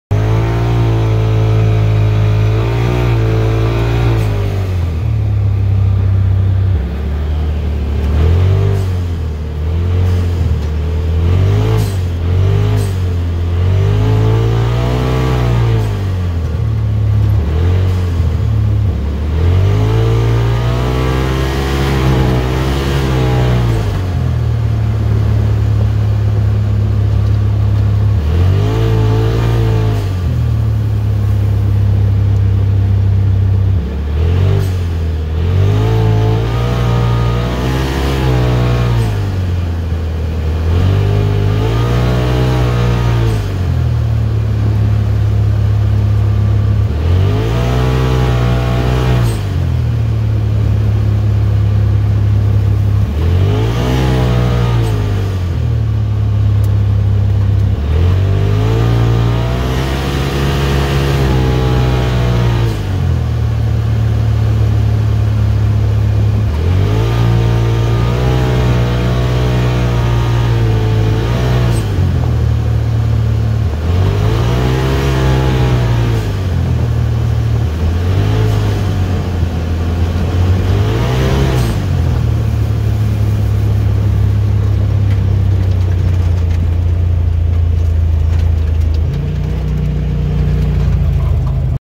RedDirtWarriors Game Management Trail October 9th 2022
Beautiful winding trails with a bit of color still evident.